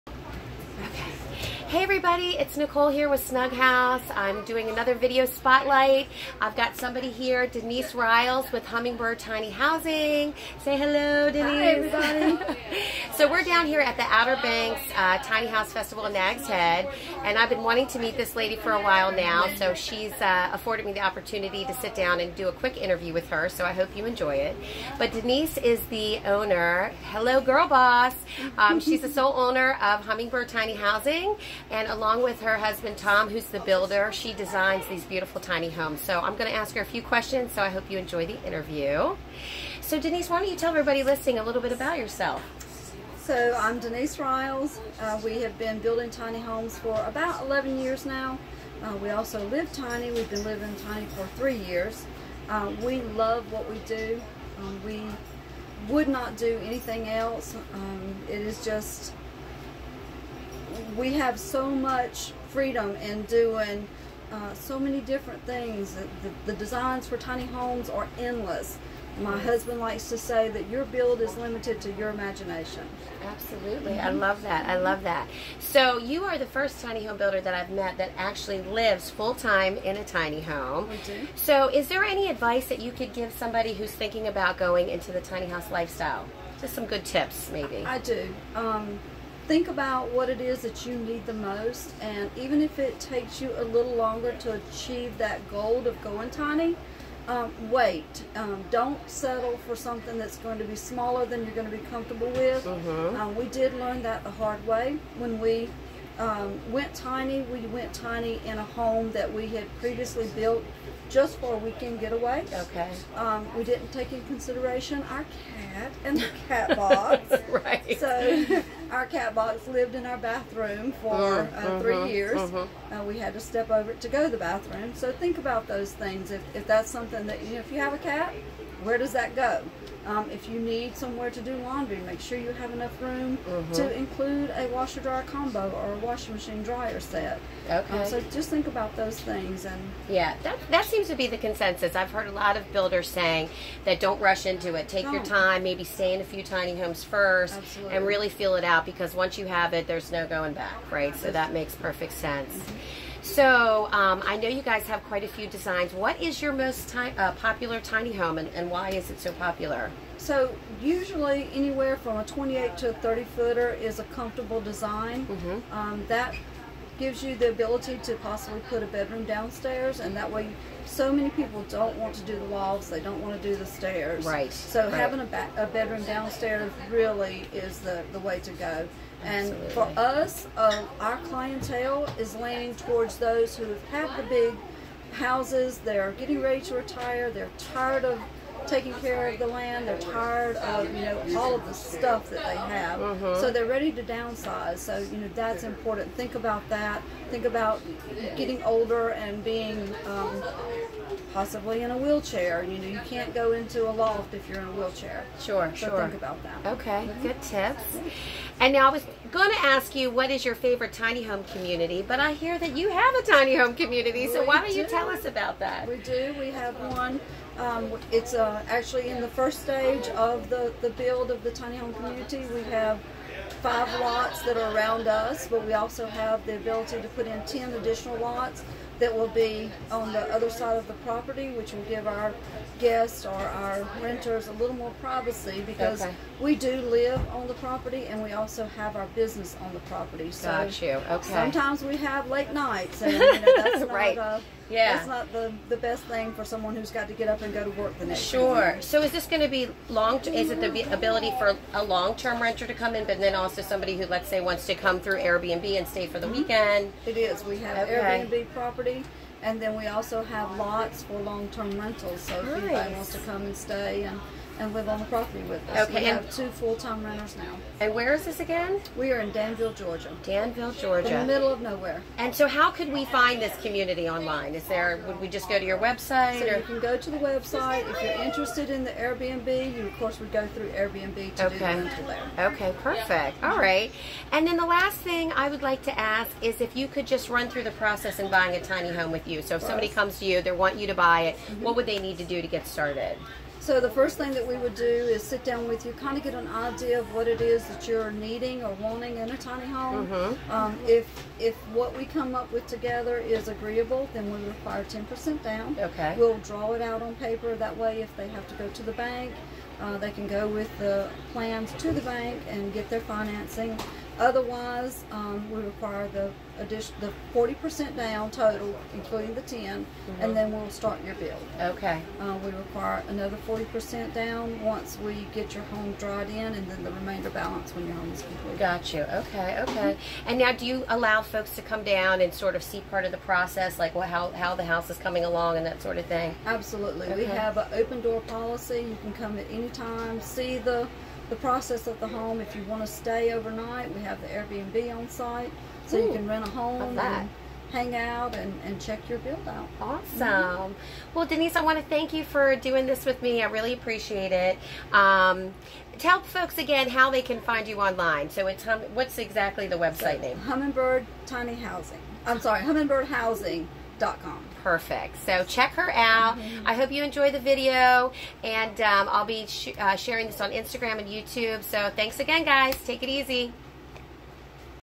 Snug Houz Video Spotlight with Hummingbird Tiny Housing
Interview with owner Denise Ryals